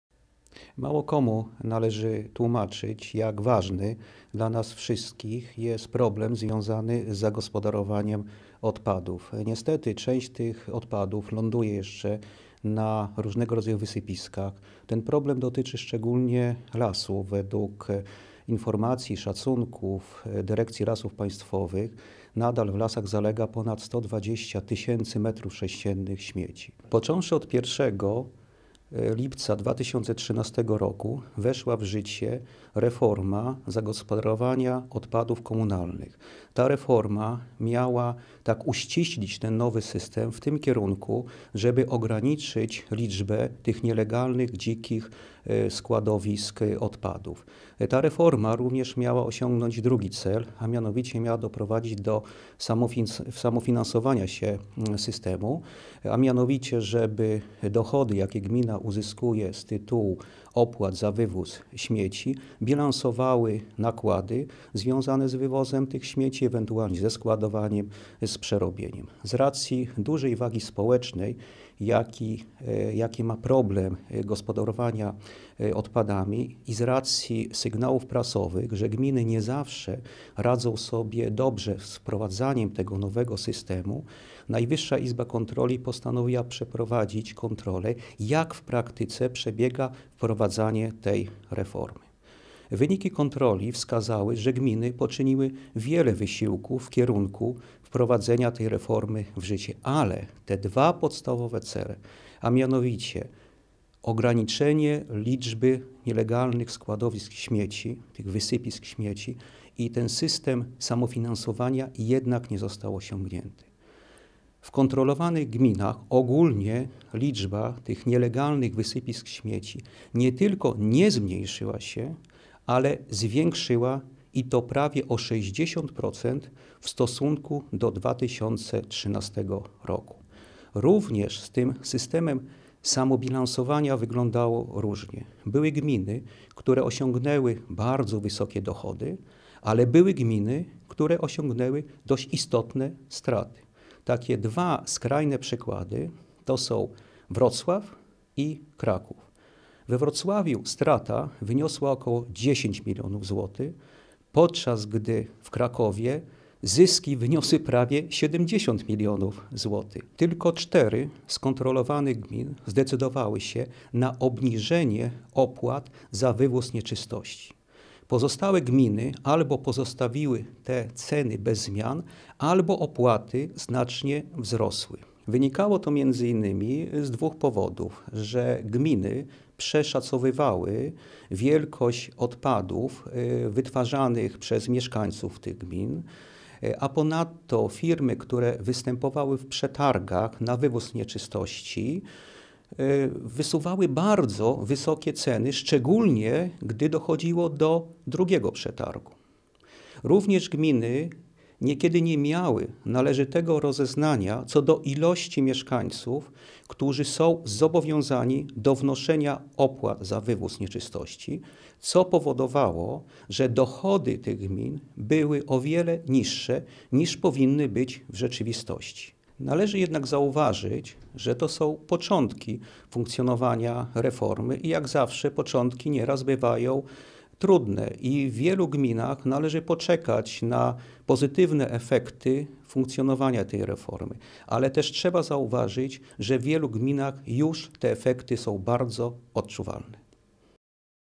NIK o odpadach komunalnych
Wprowadzony dwa lata temu nowy system gospodarowania odpadami komunalnymi nie doprowadził do rozwiązania problemu „dzikich wysypisk”, których przybywa zamiast ubywać. Rosną także koszty gospodarowania odpadami, bo gminy nie potrafią rzetelnie oszacować ilości wytwarzanych śmieci i asekuracyjnie nakładają na mieszkańców wyższe opłaty. Poza tym z firmami odbierającymi śmieci rozliczają się ryczałtem, niezależnie od ilości wywożonych nieczystości. W efekcie w nowym systemie mieszkańcy płacą więcej, choć często rzeczywiście mogliby mniej.